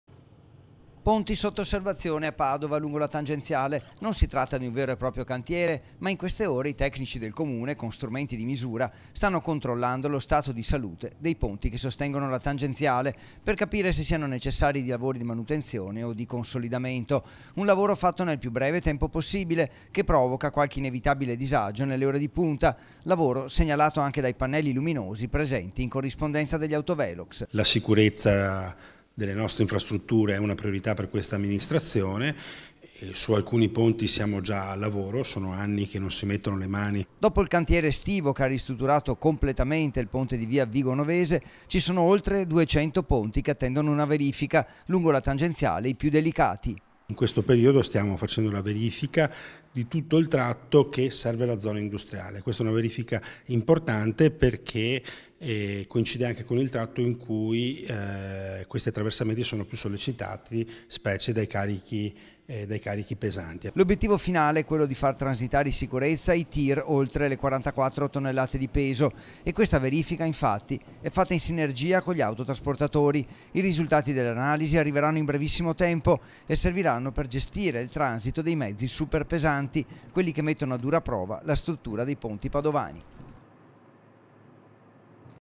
19/11/2019 | TANGENZIALE, VERIFICHE SUI PONTI
TG Padova Sera 19/11/2019 - PADOVA - Sono oltre 200 i ponti lungo le strade di Padova. Ora per quelli della tangenziale c'è la necessità di capire se siano sicuri e quali interventi di manutenzione sono necessari. Ed è partita la verifica.  || Ponti sotto osservazione a Padova, lungo la tangenziale. Non si tratta di un vero e proprio cantiere, ma in queste ore i tecnici del Comune, con strumenti di misura, stano controllando lo stato di salute dei ponti che sostengono la tangenziale, per capire se siano necessari lavori di manutenzione o di consolidamento. Un lavoro fatto nel più breve tempo possibile, che provoca qualche inevitabile disagio nelle ore di punta, lavoro segnalato anche dai pannelli luminosi presenti in corrispondenza degli autovelox.Dopo il cantiere estivo che ha ristrutturato completamente il ponte di via Vigonovese, ci sono oltre 200 ponti che attendono una verifica. Lungo la tangenziale i più delicatiL'obiettivo finale è quello di far transitare in sicurezza i tir oltre le 44 tonnellate di peso, e questa verifica infatti è fatta in sinergia con gli autotrasportatori. I risultati delle analisi arriveranno in brevissimo tempo, e serviranno per gestire il transito dei mezzi superpesanti, quelli che mettono a dura prova la struttura dei ponti padovani. - Intervistati ANDREA MICALIZZI (Assessore Lavori Pubblici Comune di Padova) - Servizio Federico Fusetti, immagini Francesco Rataj, montaggio Redazione Medianordest - Segui Telenordest anche sul digitale terrestre! Visita il sito